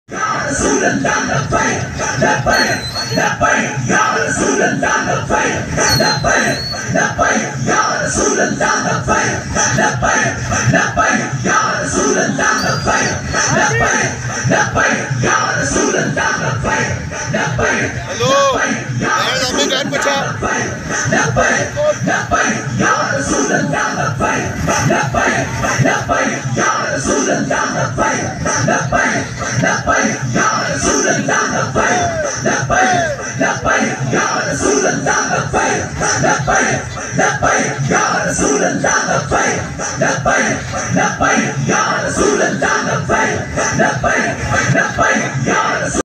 قافلہ عشق ومستی پنجاب میں داخل ہوگیا ہےحافظ سعد حسین رضوی کارکنان کے ساتھ پیدل چل رہے ہیں..
قافلہ عشق و مستی پنجاب میں داخل ہوگیا ہے قائد ملت اسلامیہ حافظ سعد حسین رضوی کارکنان کے ساتھ پیدل چل رہے ہیں..